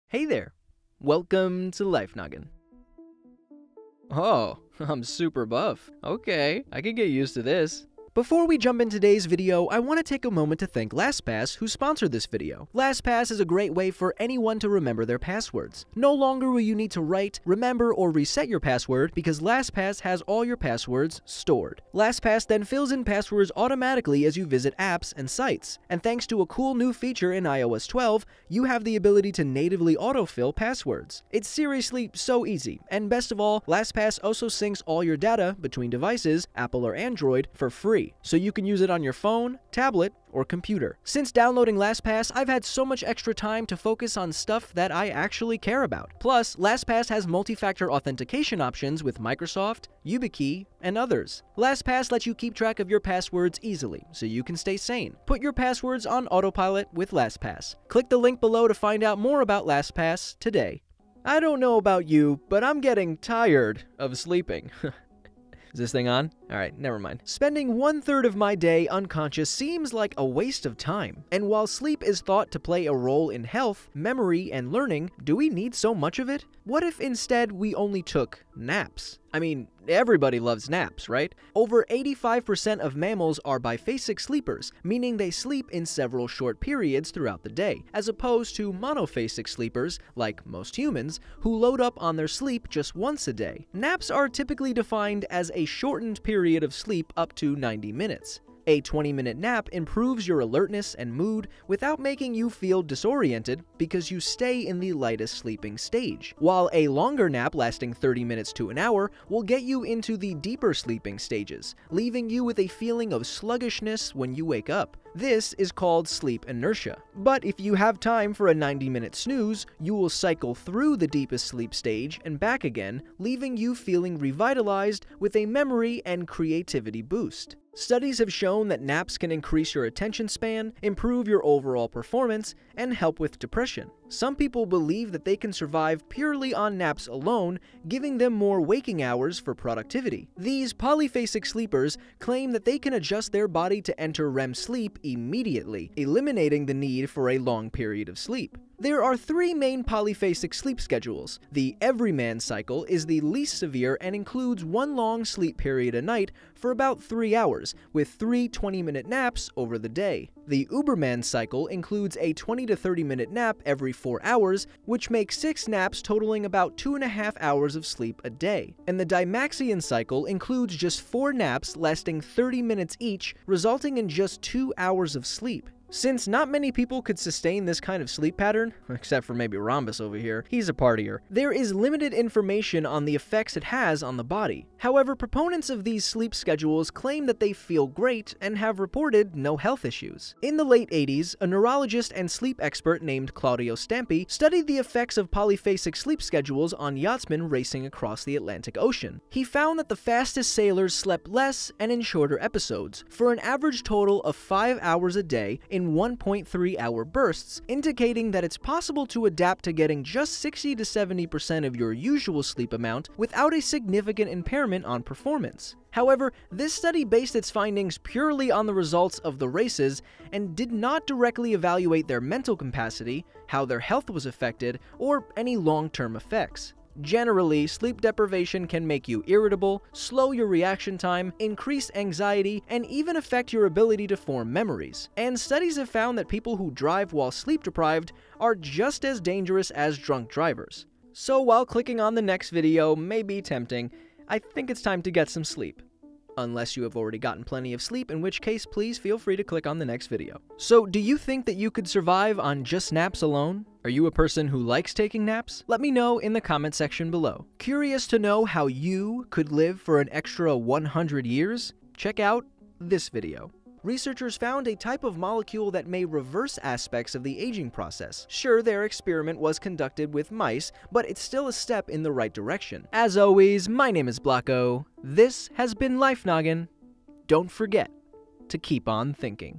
What If You Only Took Naps?
Just what exactly would happen to your body if instead of sleeping you decided to only take naps. To create a LastPass account for free & support Life Noggin, go